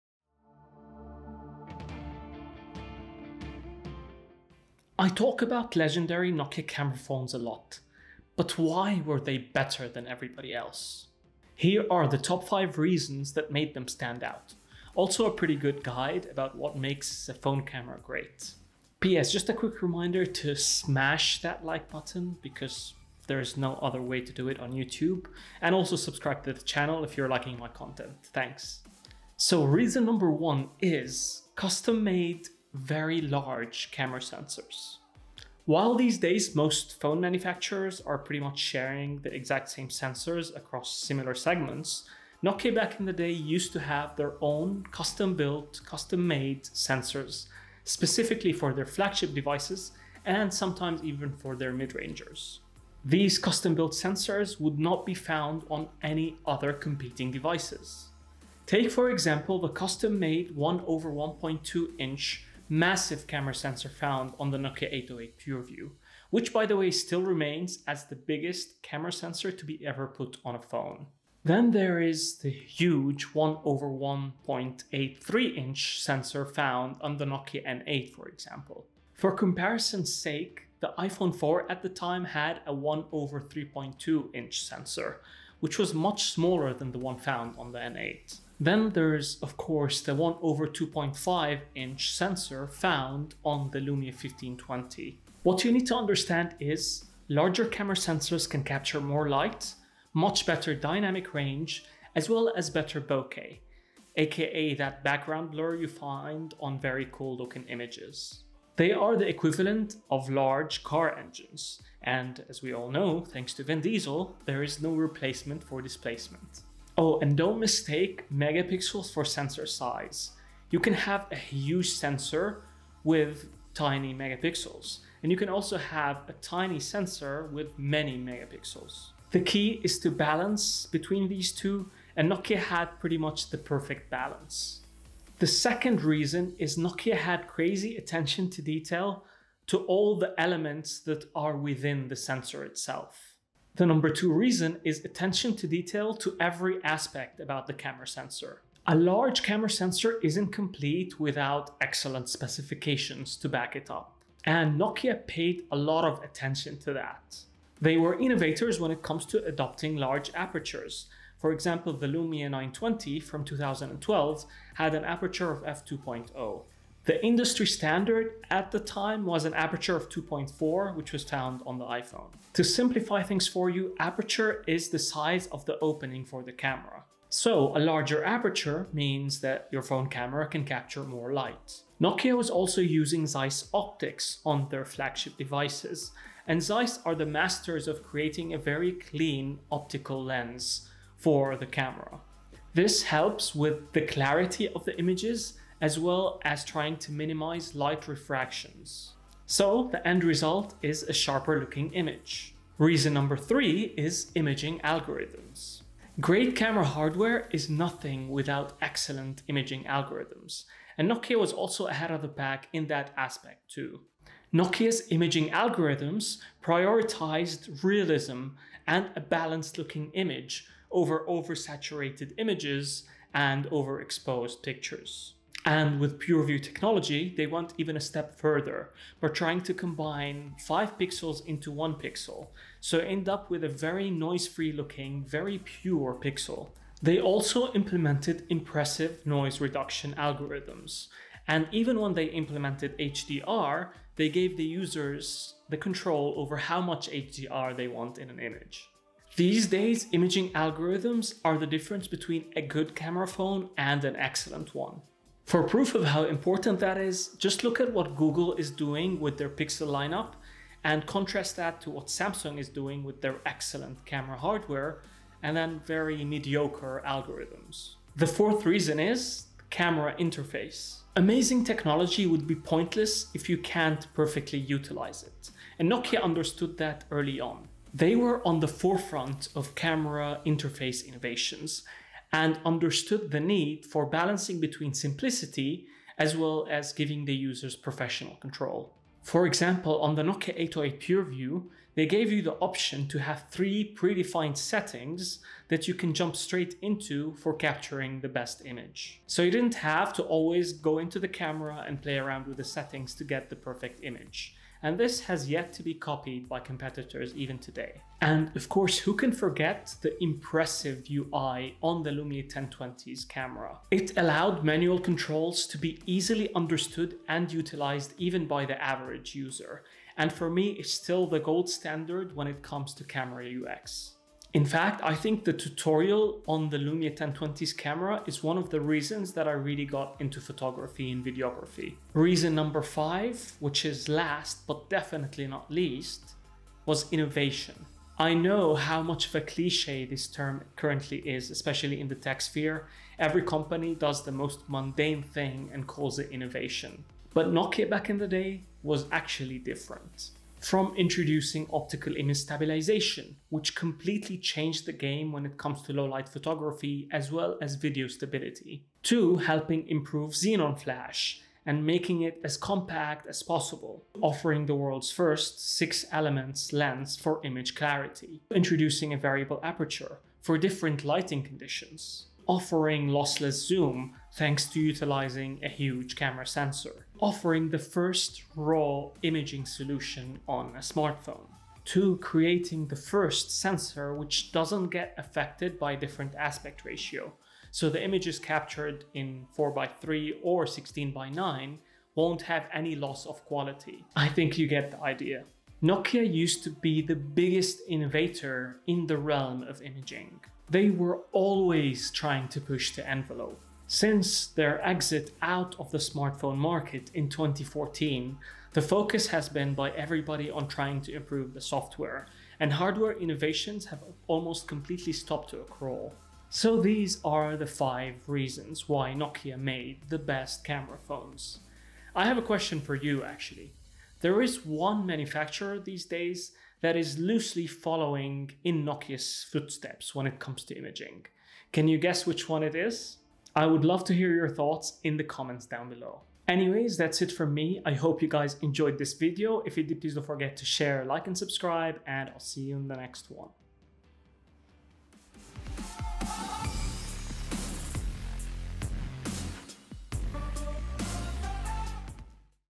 Why Nokia Had The Best Phone Cameras | The Secret Sauce
In this video I talk about the 5 reason Nokia used to have the best camera phones on the market. I talk about phones such as the Nokia N8, Lumia 1020, Nokia 808 PureView, Lumia 920, Lumia 925, Lumia 1520 and more.